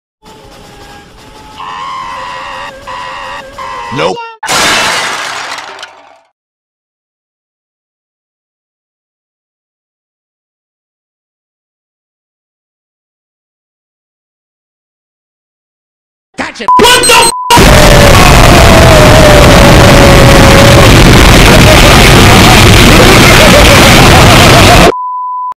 YTP Wilkins Coffee Collab Round 24 Deleted Scene: I throw a Hammer at Viacom
This is what I was aiming for In Round 24 of the Wilkins Coffee YTP Collab. But the online photo editor didn't work for me at the time.
Note: I didn't add my watermark. That was my old watermark. Aka what I used to do before I became a video editor and a YTP maker.